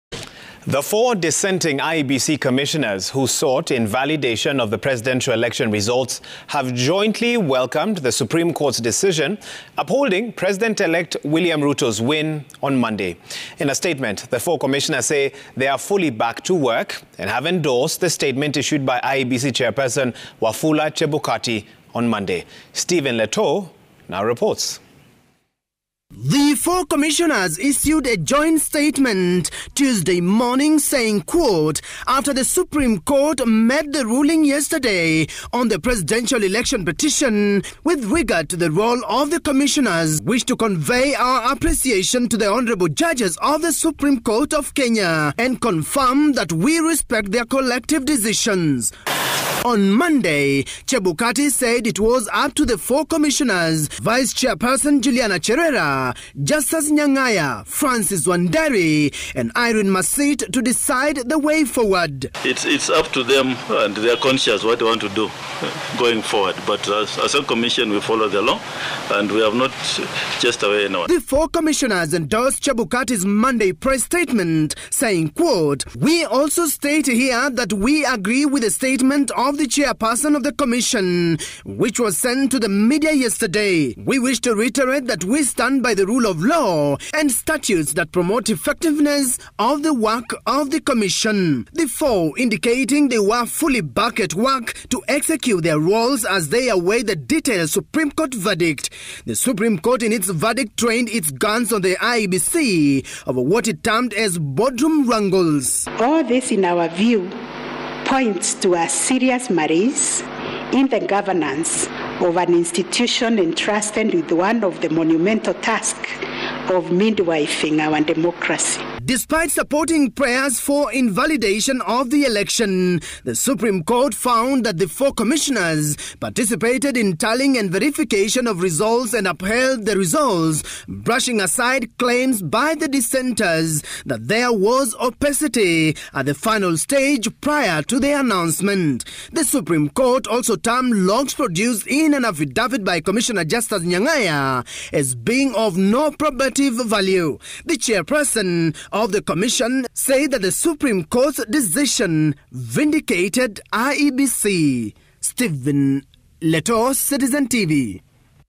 'IEBC 4' commissioners say they accept Supreme Court verdict
The four dissenting IEBC commissioners who sought invalidation of the presidential election results have jointly welcomed the Supreme Court’s decision upholding President Elect William Ruto's win on Monday. In a statement the four commissioners say they are fully back to work and have endorsed the statement issued by IEBC Chairperson Wafula Chebukati on Monday.